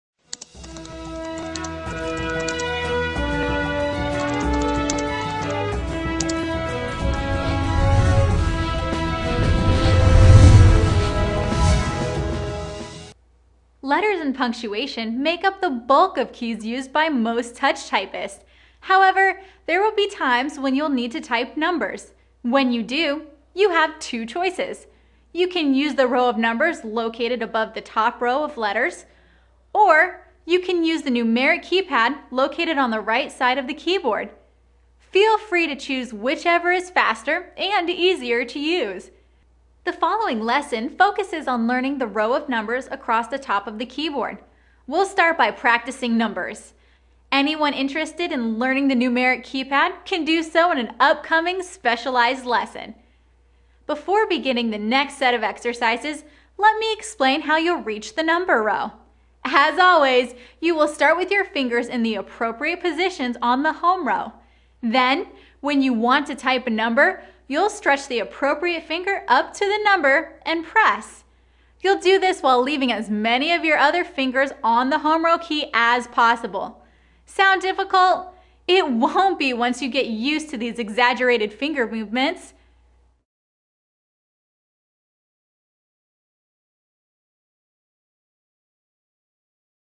Learn How To Type Numbers
Depending on what you type, you might not use the number keys very often, but it's important to know where they are and how to type them. What's more, a good mastery of the number keys is crucial if you want to maximize your efficiency in a job where typing is a primary part of your day.